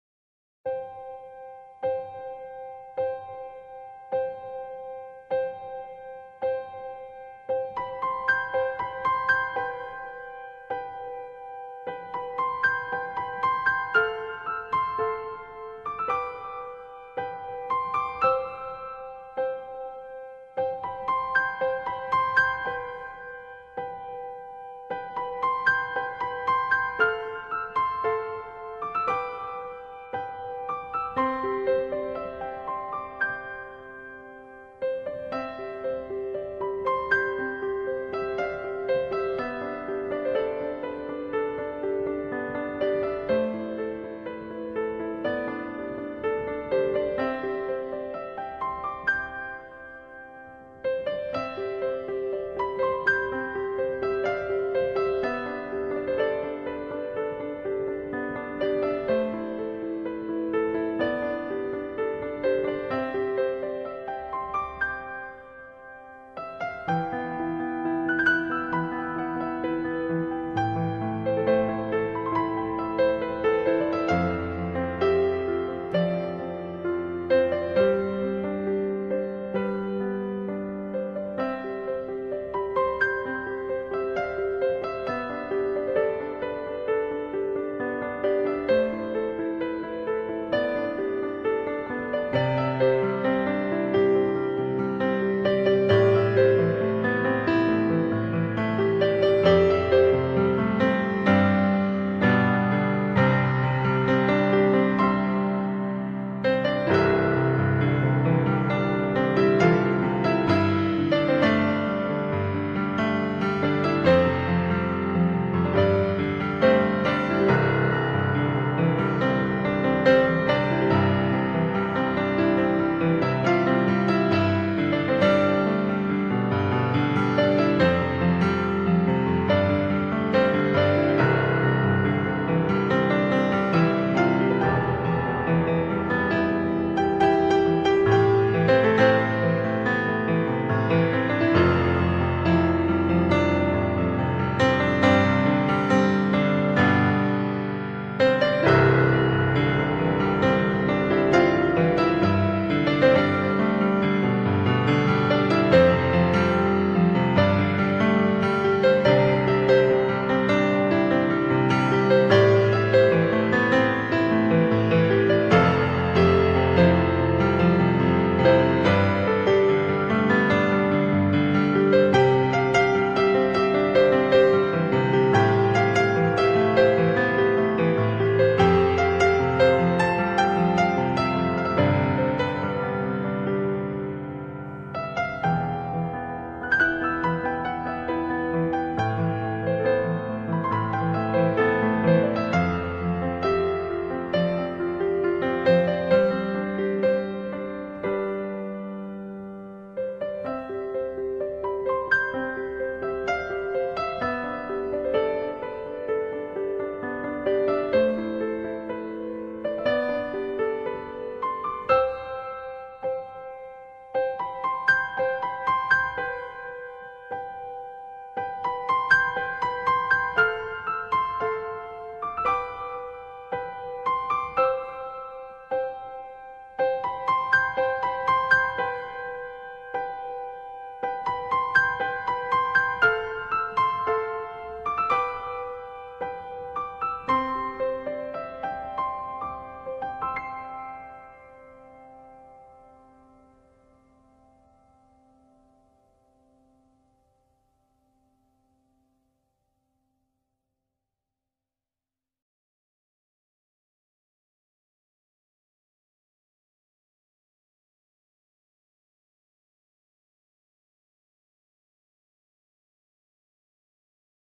Autism/Special Education Flashcard CD-ROM
This new CD was developed to teach children with Autism, PDD and other learning delays and difficulties.